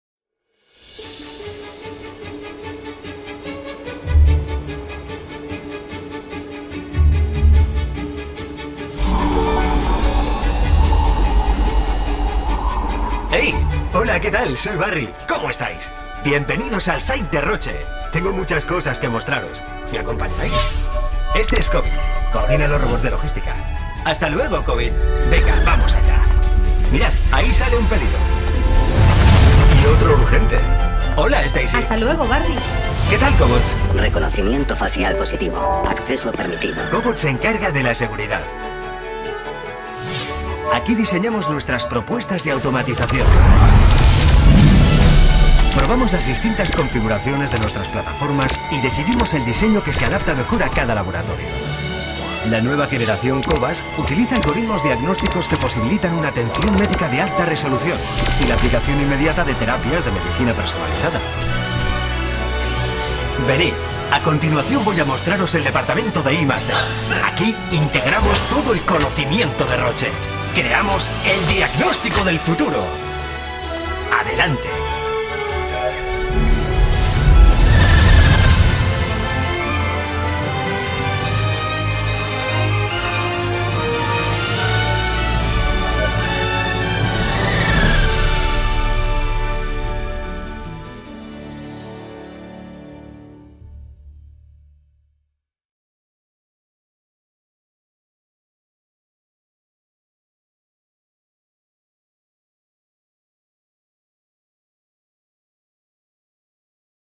2018 - ROCHE
At ROCHE Marketing department, they decide to break the rules and trust in our talent to produce a couple of Films based on SCFI and Futuristic inspiration to represent the new features of the department.